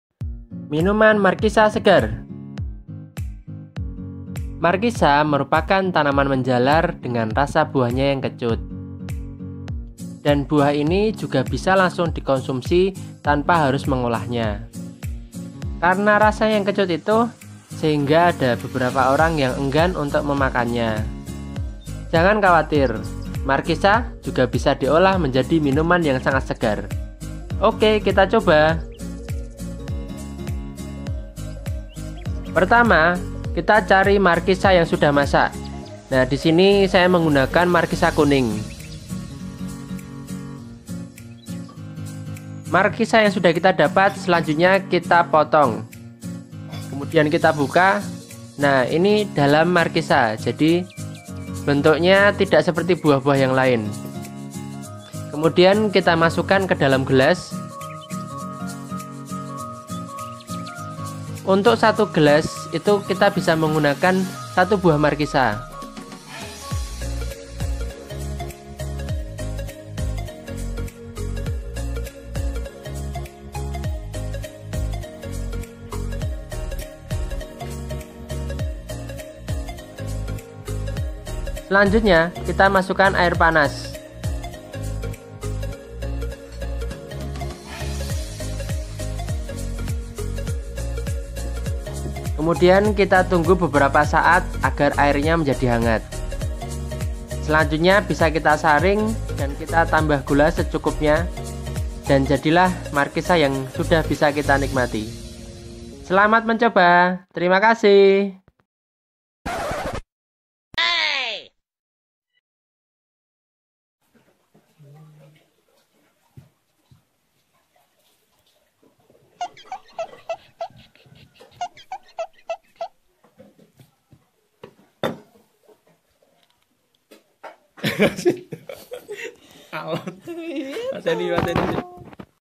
Cara Mengolah Markisa Jadi Minuman Segar | Markisa Kuning
Markisa yang rasanya asam / kecut menjadikan beberpa orang enggan untuk mengkonsumsinya. Tanaman markisa ternyata juga memiliki warna dan jenis yang berbeda-beda. Ada yang buahnya berwarna kuning, Merah, ungu, dan markisa sayur.

Pada video kali ini,  yuktani com akan mempraktekkan cara membuat minuman dari buah markisa.

Bisa diminum saat hangat, bisa juga d tambah es, bisa juga d tambah sedikit teh.

Selamat mencoba